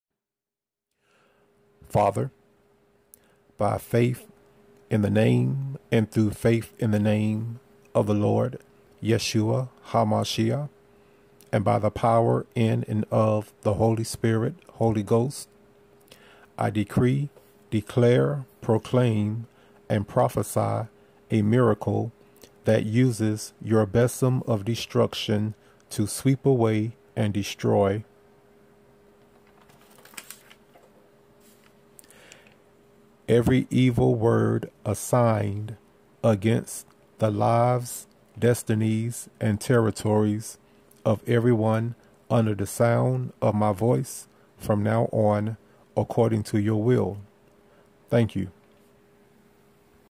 Besom Of Destruction Sweeps Away & Destroy Evil Assigned Word Prayer🧹🙏🔥🏃‍♂️🏃🛐✝️😇
This Miraculous Declaration is based on Isaiah 14:23 kjv- "I will also make it a possession for the bittern, and pools of water and I will sweep it with the besom of destruction, saith the Lord of Hosts